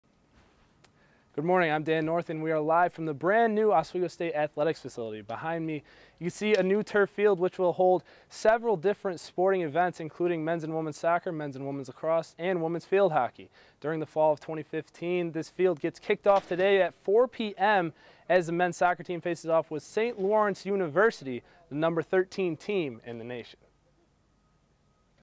Oswego State Athletic Field
A stand up introducing SUNY Oswego's brand new turf stadium.